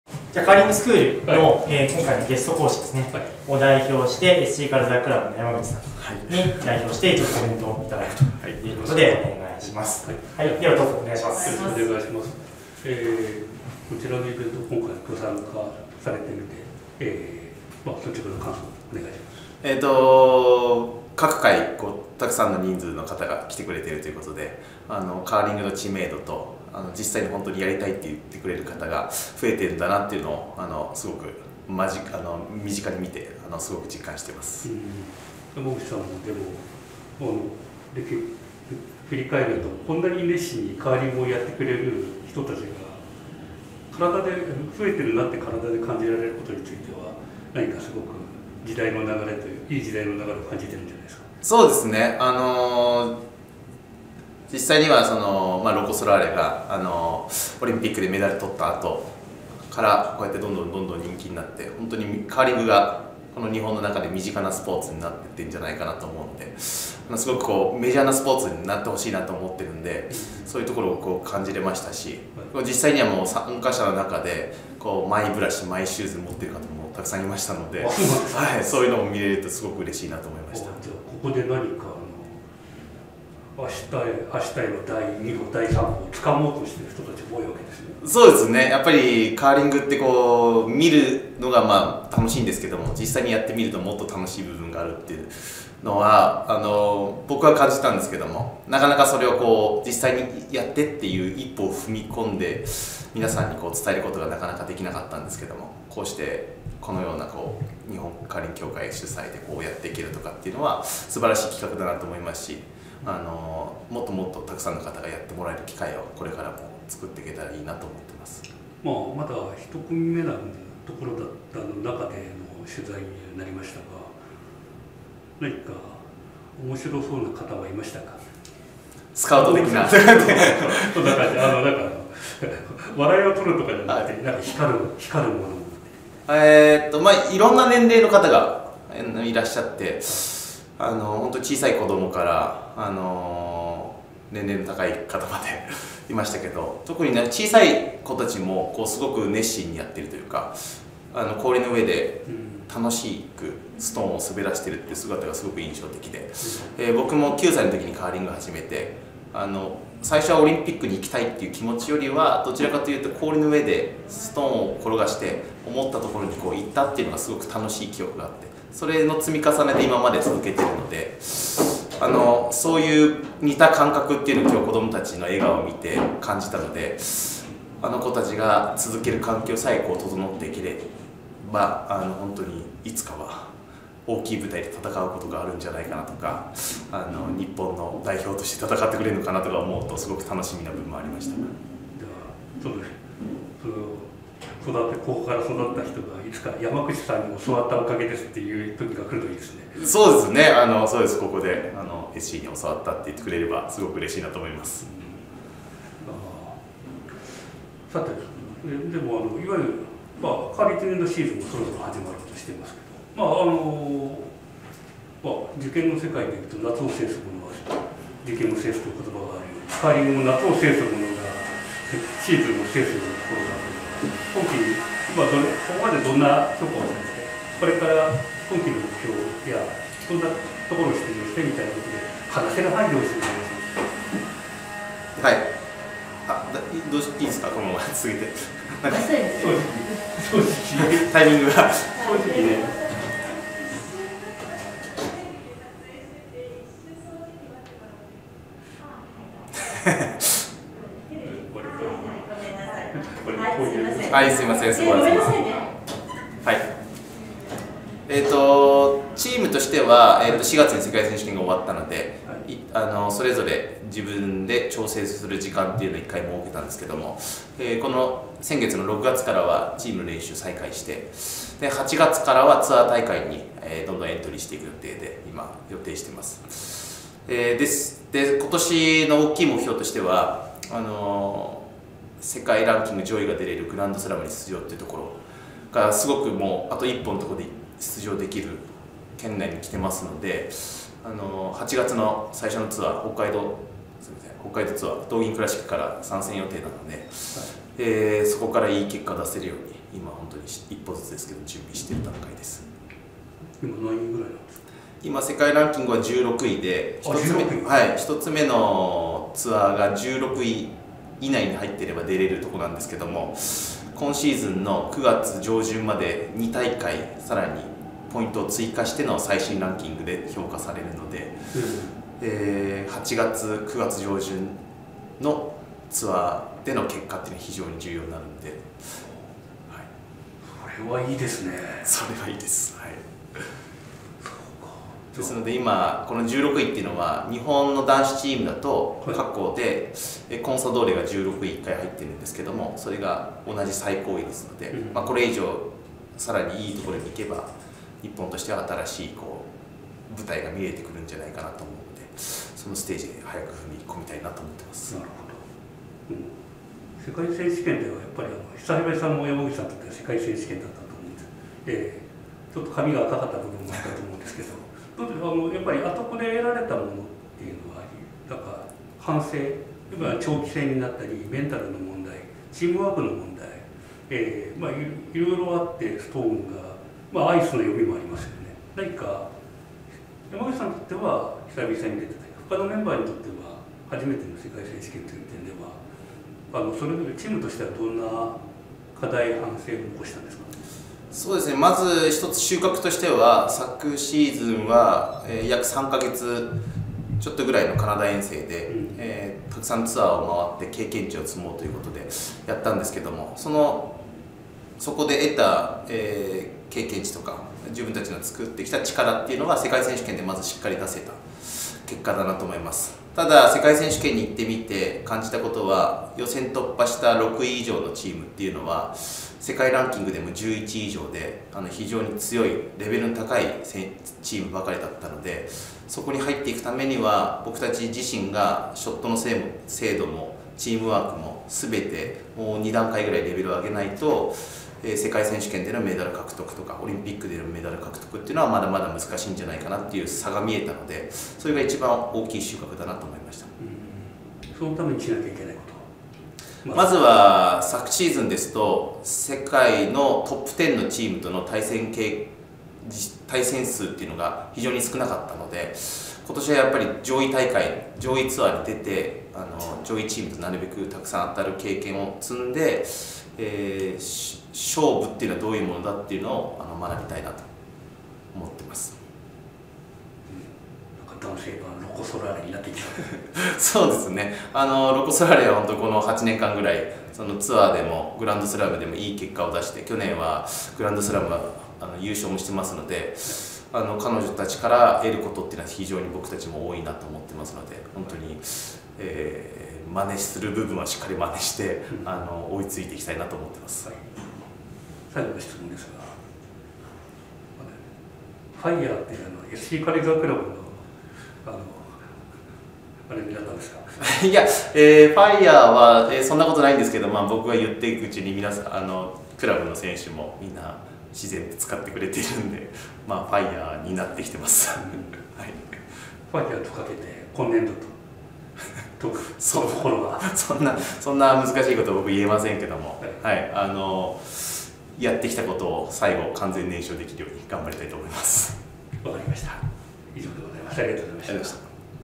大盛況！横浜でカーリング教室開催 小中学生含む168人が参加(2023年7月4日)
7月2日、「JCAカーリングスクール横浜2023」が横浜銀行アイスアリーナで開催されました。
このカーリング教室は毎年夏に行われ、横浜で4回目を迎えました。
今回は普及活動の一環で一般参加者を募集。
小中高校生40人と一般128人、合計168人が参加し、
SC軽井沢クラブの男女チームが中心となって指導にあたりました。
特に小学校低学年はストーンに触れて投げることなど体験をベースに。
小学4年以上は投げやスイープ、基本ルールなどを実地指導。
みなたどたどしくも楽しそうに体験していました。
ゲスト講師として参加した、平昌五輪代表・山口剛史選手に話を聞きました。 ※この映像にはナレーションはありません。ご了承ください。